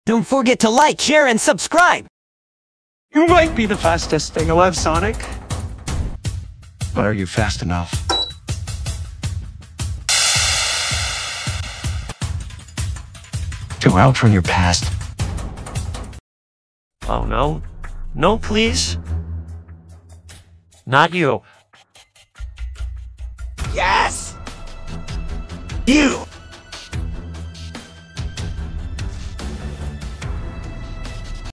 Sonic's Worst Enemy (Sonic Comic Dub)
Sonic faces off against Robotnik, unfortunately, he's brought back up with him and it's someone that Sonic had hoped he'd never see again. Ugly Sonic is back and it looks like he wants to have a chat with the new movie Sonic.